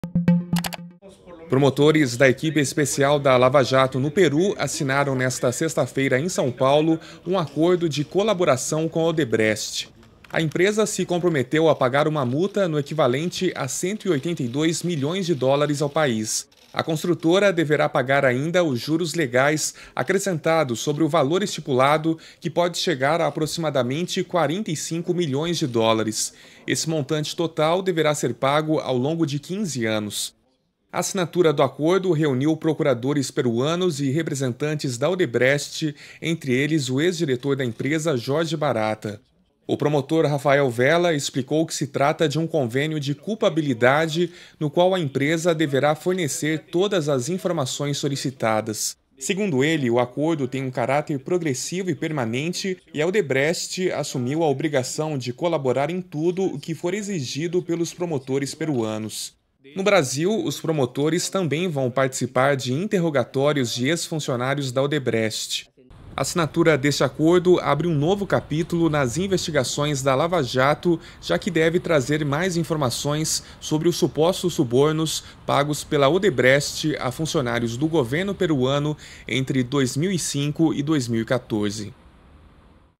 Odebrecht fecha acordo para pagar US$ 182 milhões ao Peru por subornos
São Paulo, 15 fev (EFE),(Imagens: Alex Mirkhan) - Os promotores da equipe especial da Lava Jato no Peru, Rafael Vela e José Domingo Pérez, assinaram nesta sexta-feira no Brasil um acordo de colaboração com a Odebrecht, que se comprometeu a pagar 610 milhões de soles (US$ 182 milhões) ao erário desse país.

INCLUI IMAGENS DE ARQUIVO